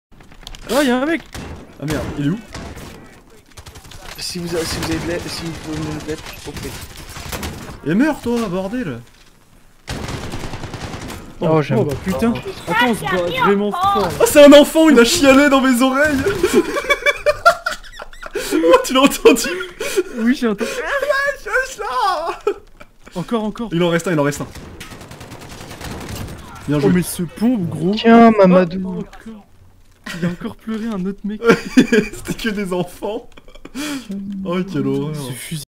Bonne nuit les petits.
Un drame terrible s'est produit dans la ville de Saint-Jeremy-Les-Bains, alors que la classe de primaire de la commune s'est faite sauvagement abattre par des soldats surentraînés qui seraient, selon certains témoins, capable d'affronter des gorilles tueurs à mains nues.